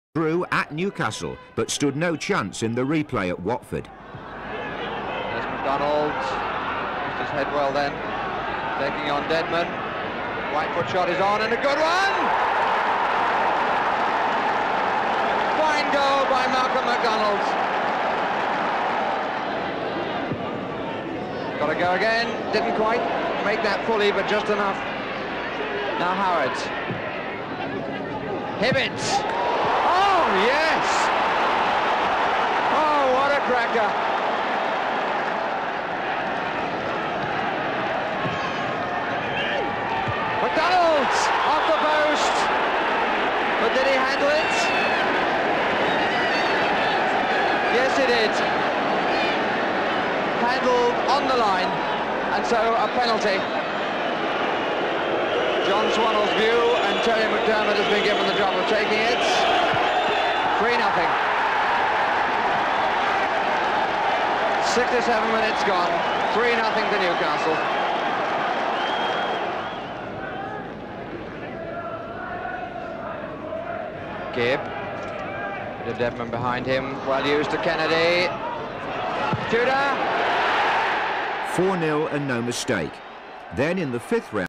Hendon v Newcastle Utd F.A. Cup 3rd Round Replay 09-01-1974
Non League Hendon had performed brilliantly at St James's Park to claim a replay. Could they finish the job at home? Game played at Watford's ground.